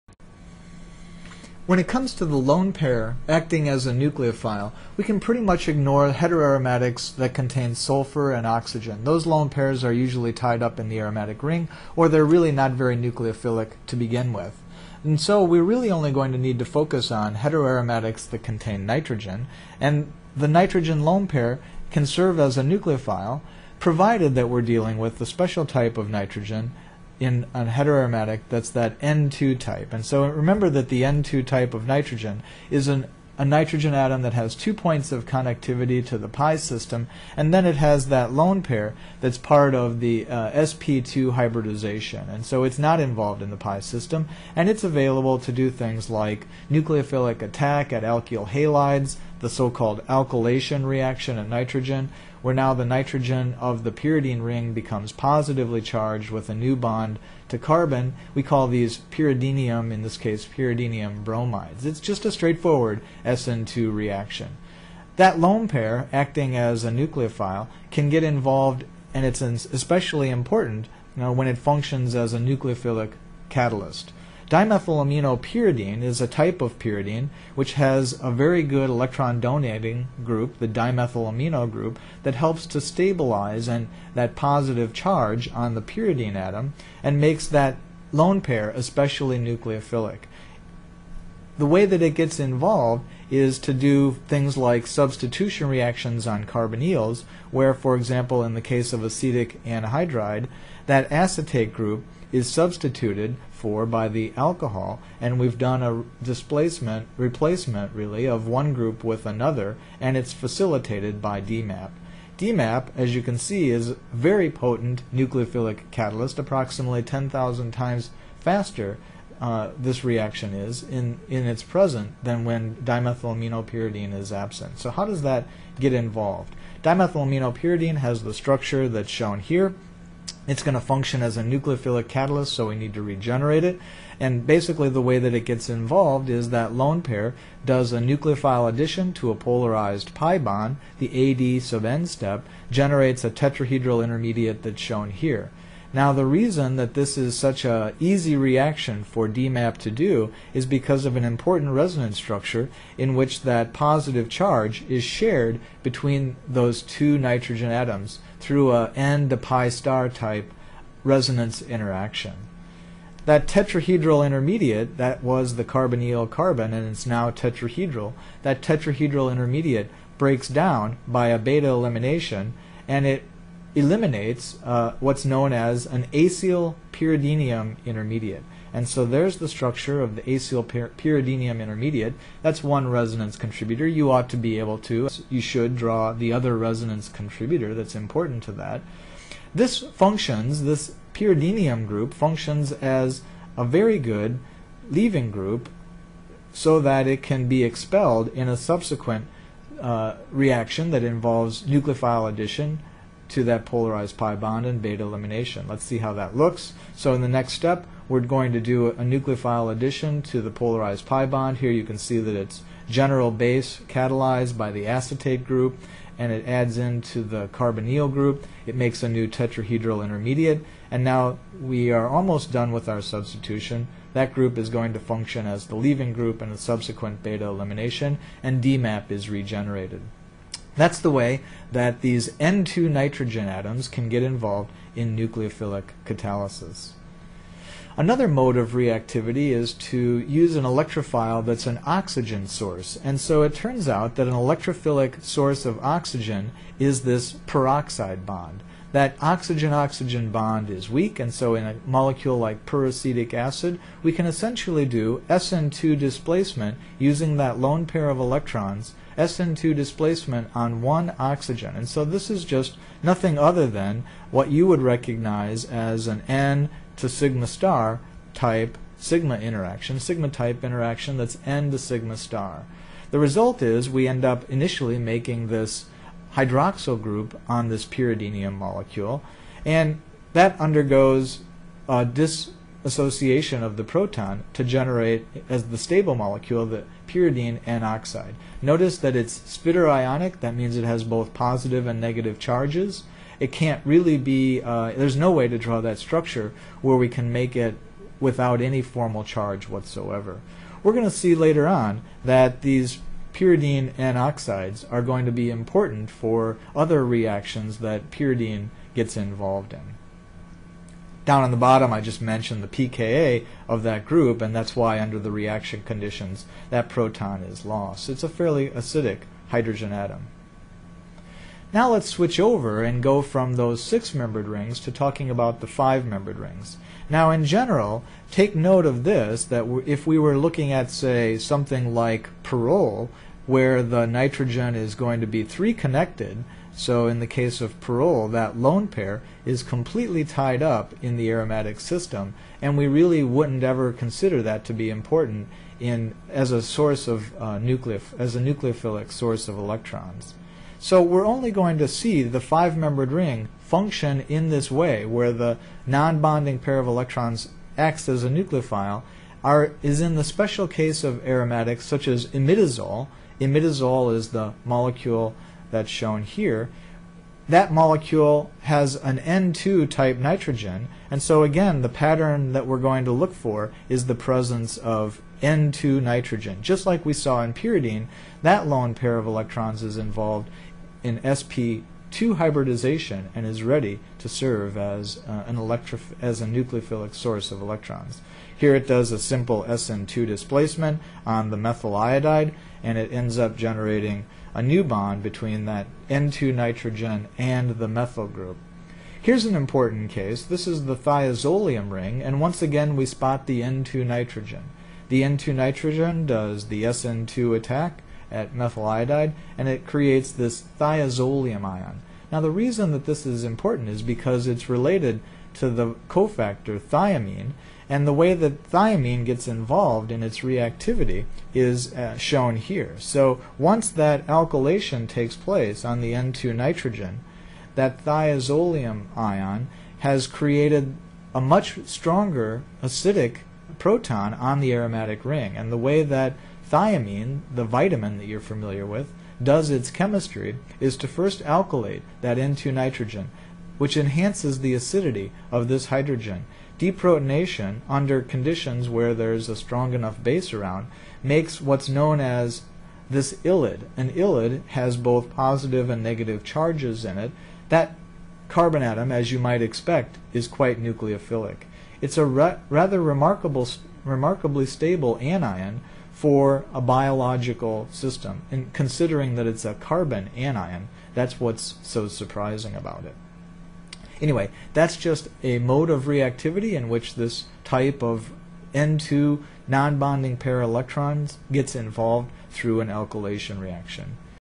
Heteroaromatics as N-Nucleophiles
What are the various ways in which nitrogen atoms in heterocycles can behave as nucleophiles?